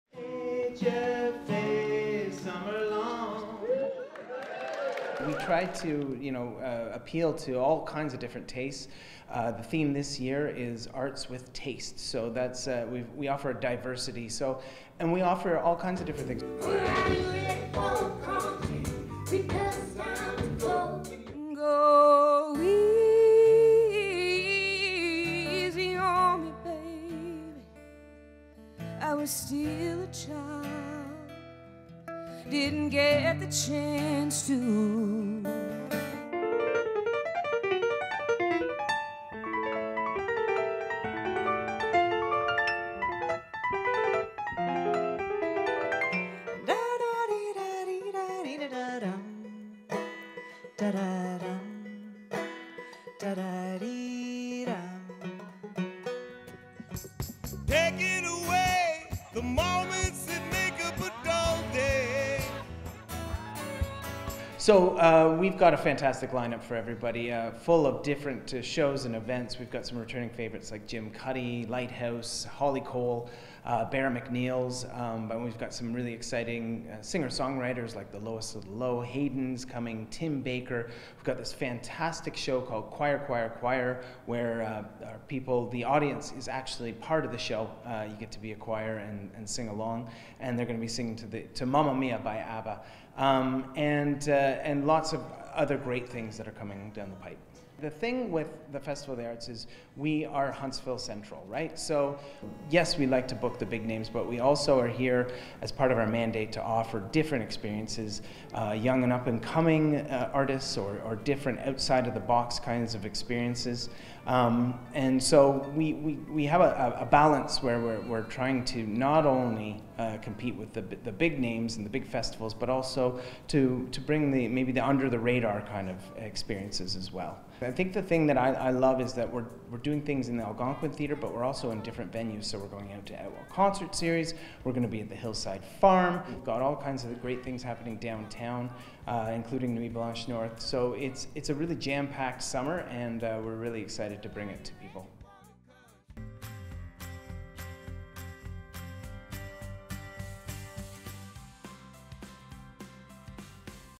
Huntsville Festival Of The Arts Launch Announcement
The Summer Line Up for Huntsville Festival of the Arts has been announced. Algonquin Theatre